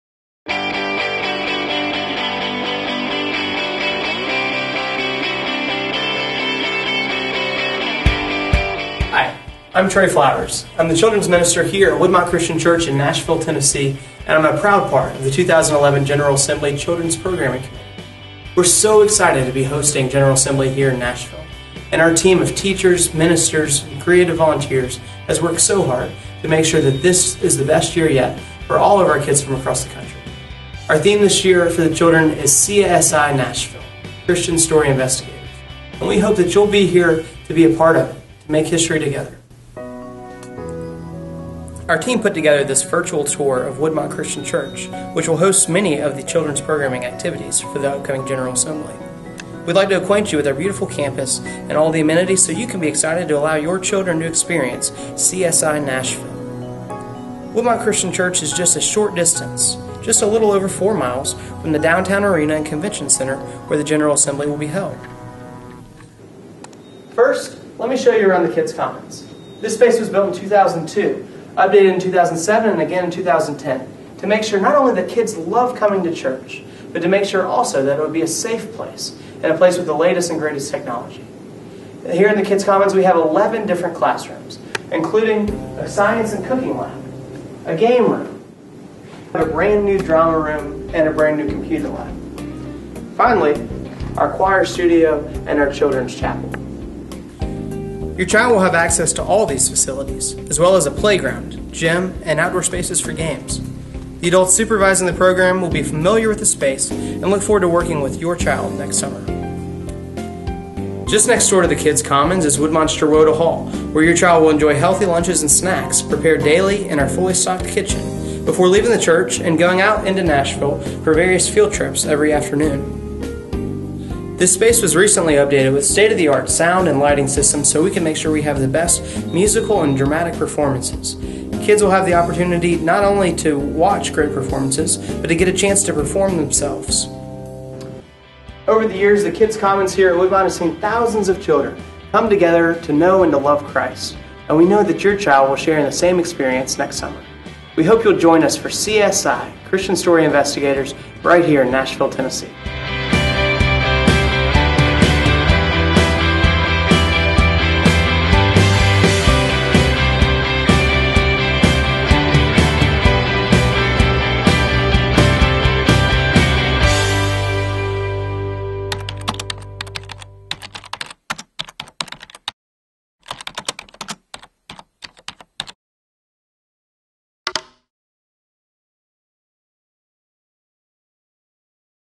Children's Program at the 2011 General Assembly
Watch a tour of the facilities and information about the 2011 General Assembly Children's Program in Nashville, Tennessee.